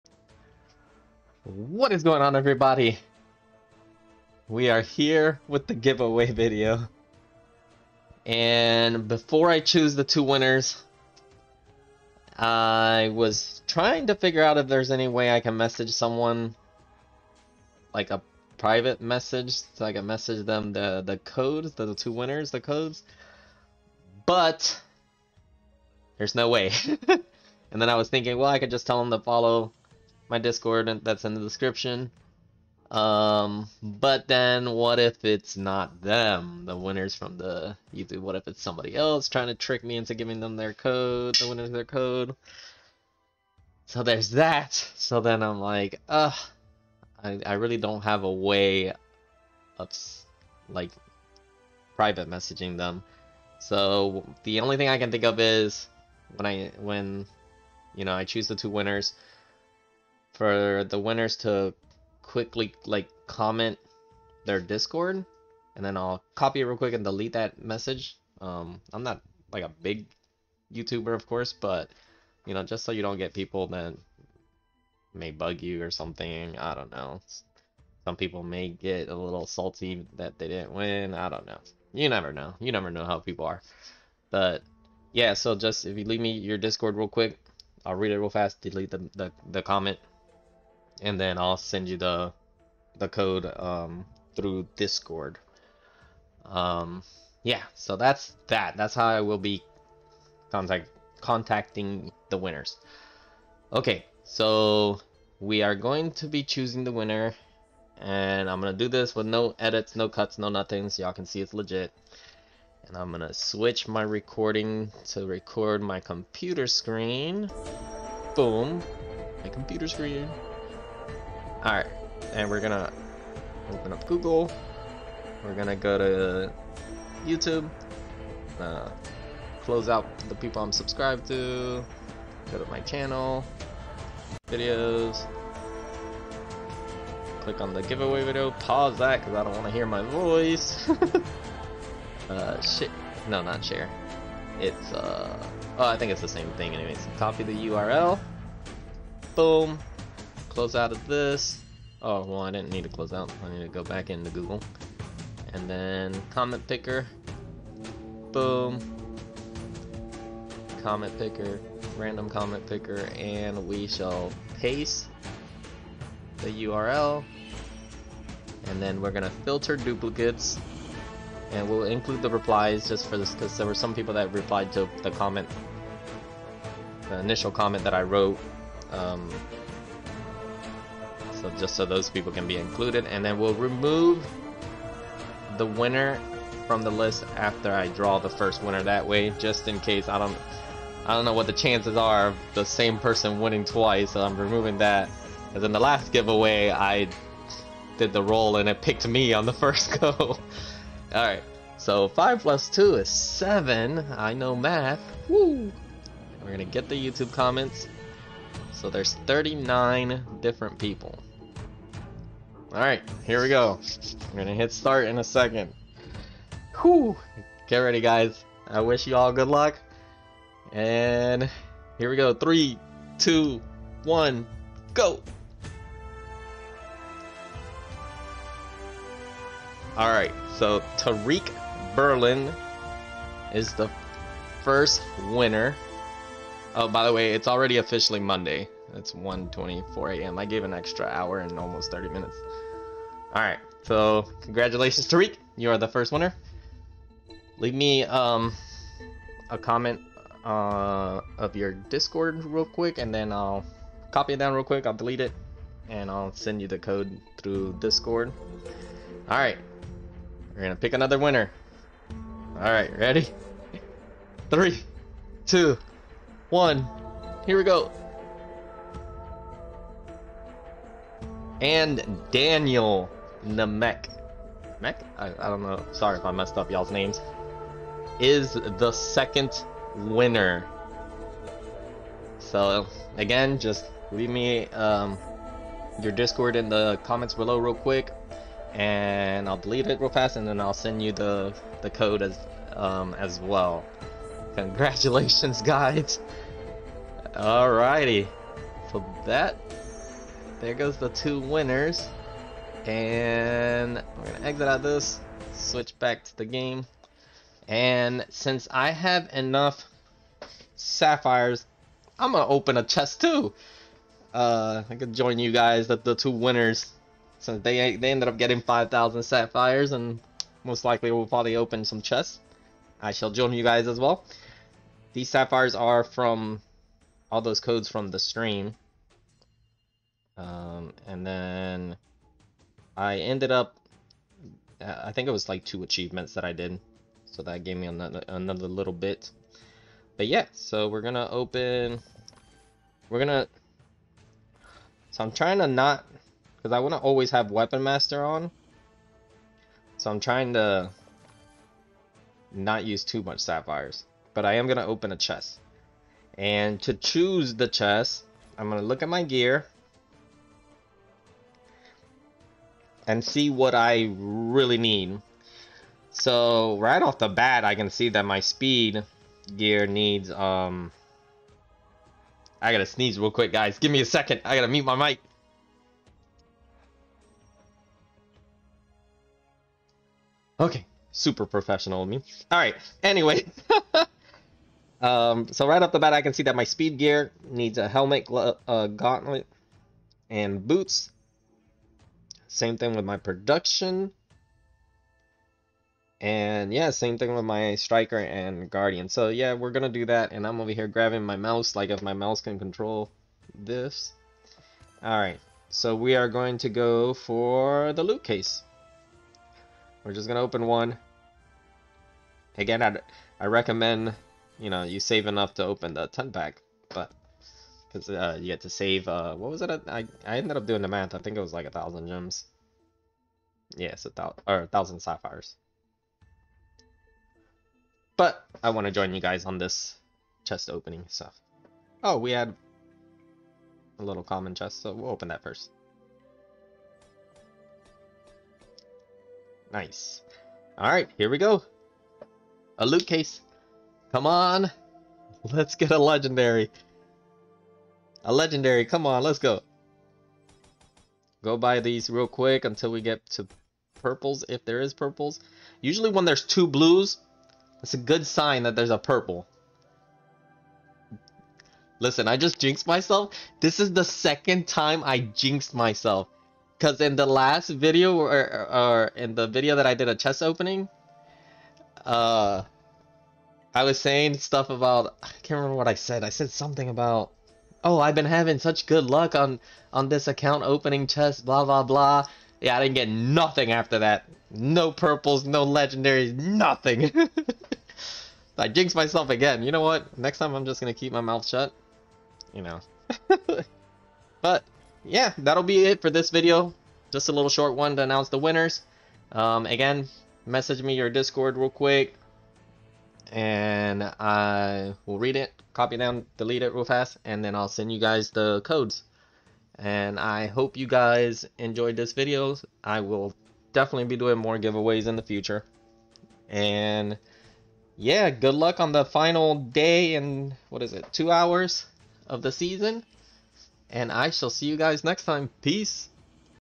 MILLION LORDS: The 2 giveaway winners are.. **READ THE DESCRIPTION ON HOW TO CONTACT ME**
So winners ignore the way I mentioned in the video since youtube seems to automatically delete those comments. I will then send you a private message on there with a keyword for you to comment in this video to make sure that is really you. Once I validate that it's you I will then send you the code on youtube. Thank you!!!

I hope you all enjoy the video! Thank you all for your support!